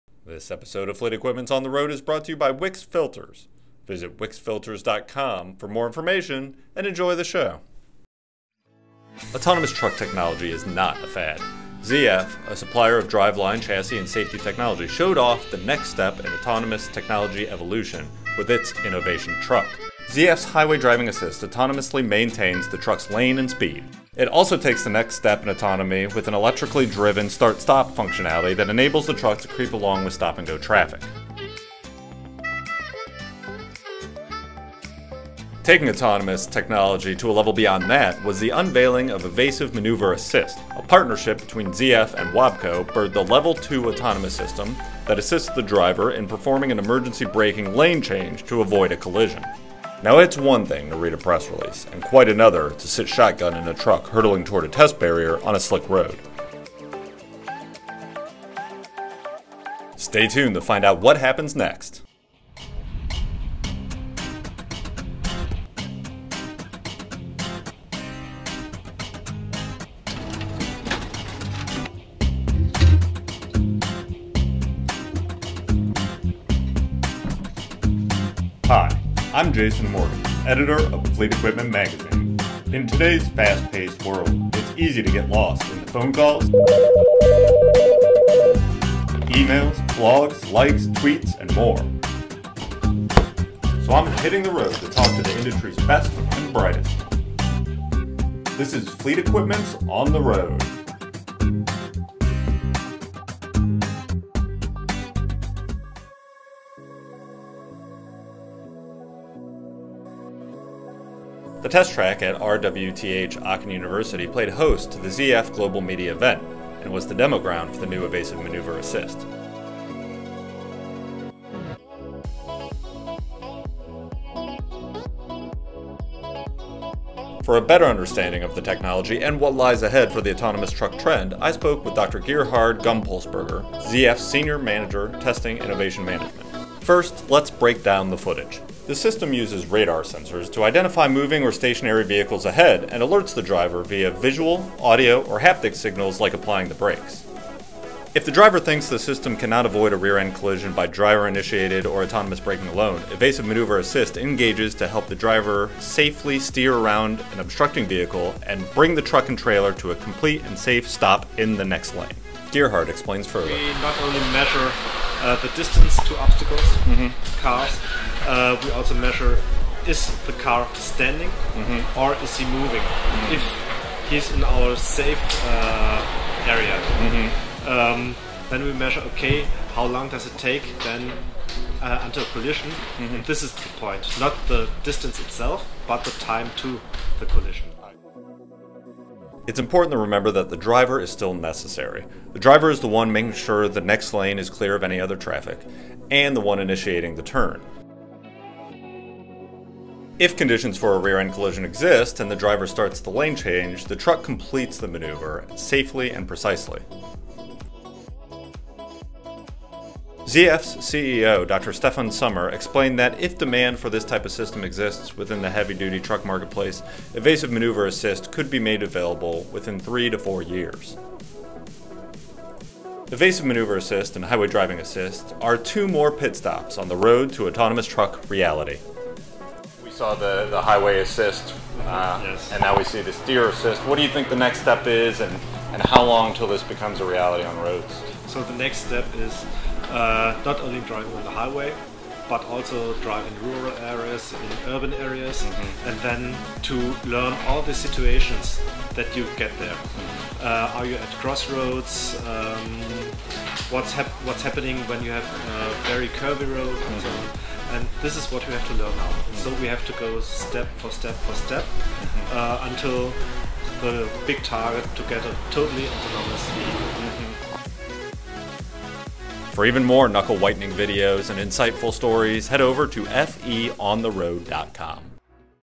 Autonomous Evasive Maneuver Assist in action—ZF Innovation Truck
Autonomous truck technology is not a fad. ZF, a supplier of driveline, chassis and safety technology, showed off the next step in autonomous technology evolution with its Innovation Truck. Taking autonomous technology to the next level was the unveiling of Evasive Maneuver Assist. A partnership between ZF and WABCO birthed a level two autonomous system that assists the driver in performing an emergency braking lane change to avoid a collision. For a better understanding of the technology and what lies ahead for the Autonomous truck trend, I spoke with Dr. Gerhard Gumpoltsberger, ZF’s Senior Manager Testing, Innovation Management.